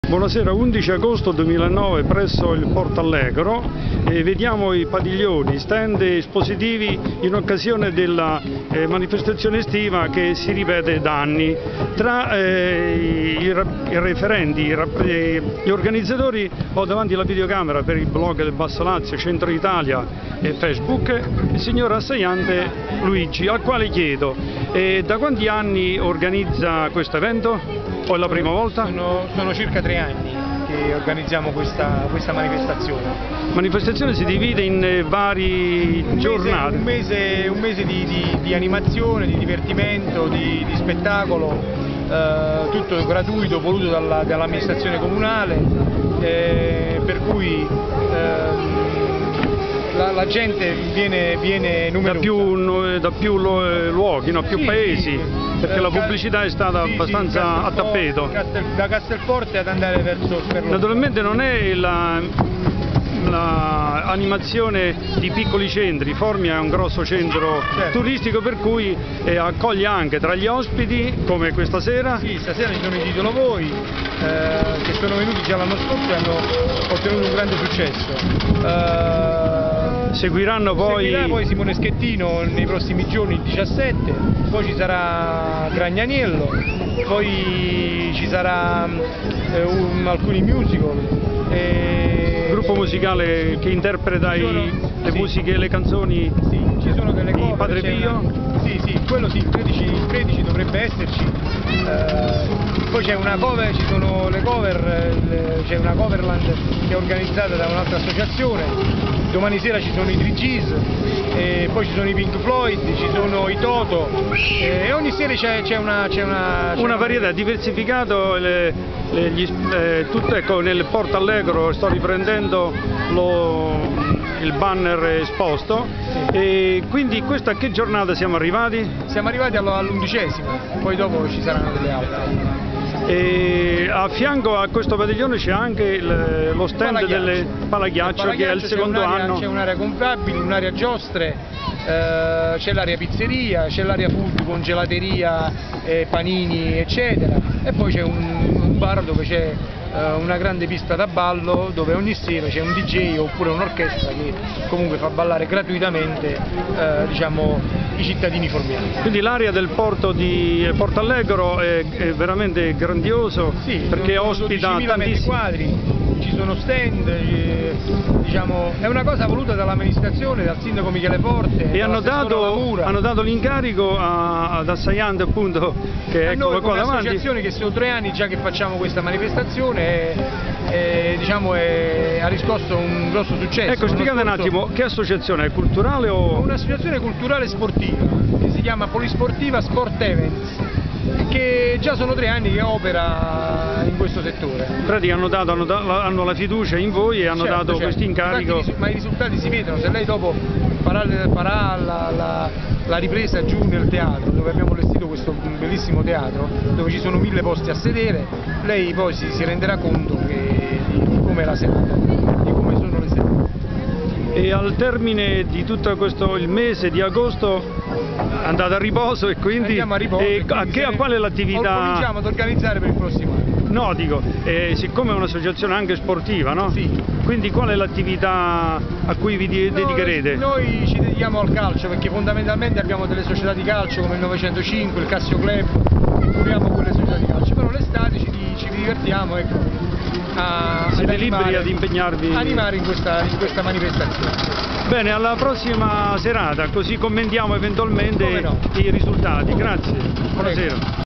Formia spettacoli,Responsabile Luigi Assaiante
Intervista al Responsabile degli EVENTI formiani, Luigi Assaiante, nel Villaggio del DIVERTIMENTO a PORTO ALLEGRO,Molo Vespucci di FORMIA(LT).Video e Intervista di Federico Galterio.